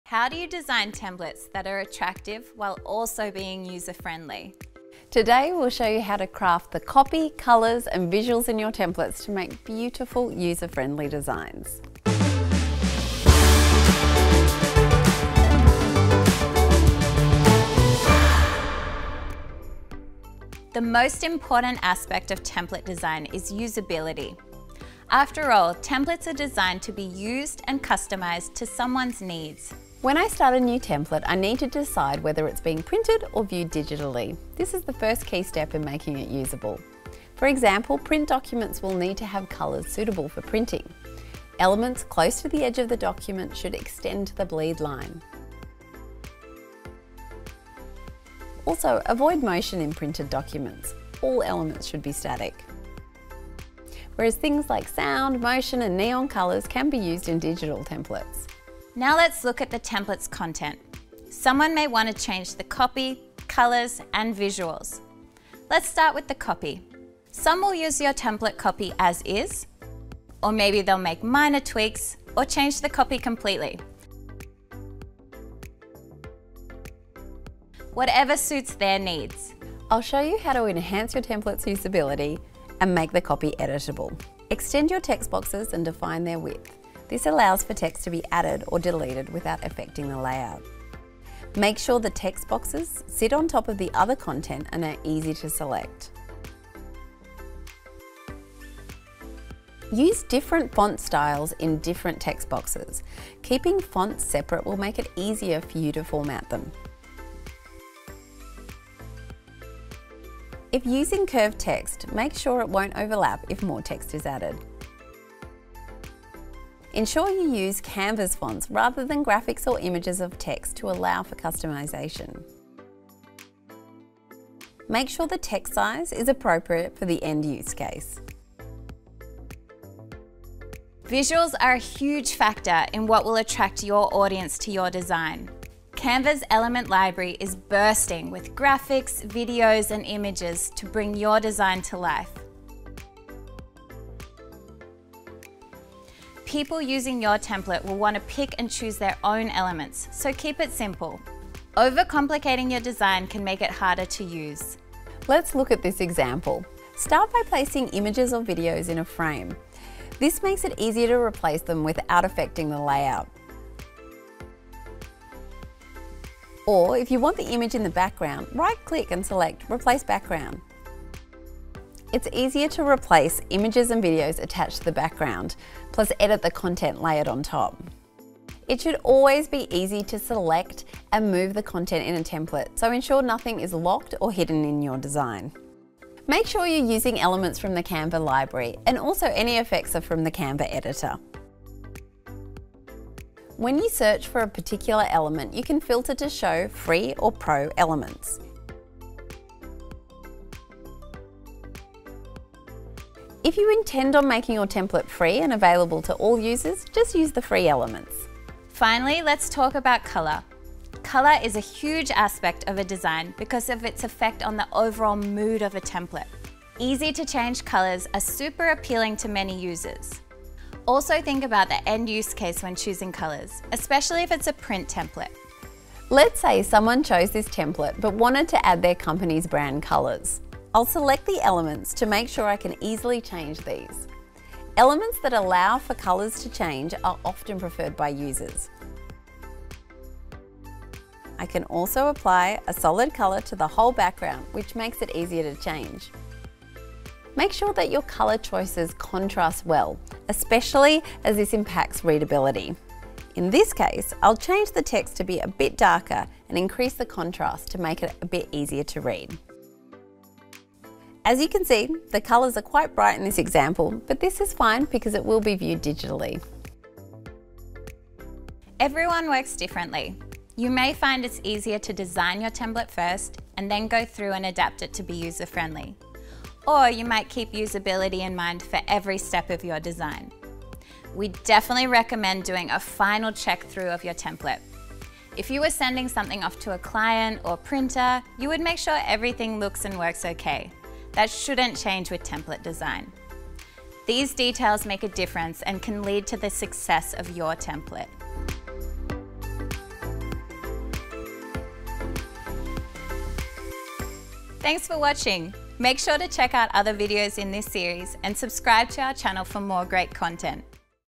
User-friendly design | Designing Templates
Template design empowers everyone to design by giving them a great starting point that they can easily edit and use for their own needs. There is so much potential in designing templates for a marketplace and Canva is the best tool for this. Learn the important steps in creating successful, user-friendly templates.

► How to design beautiful, user-friendly designs
► Crafting copy, colors and visuals to allow for customisation

In the next episode of Designing Templates, we will take you through a helpful framework to analyze the success of your templates.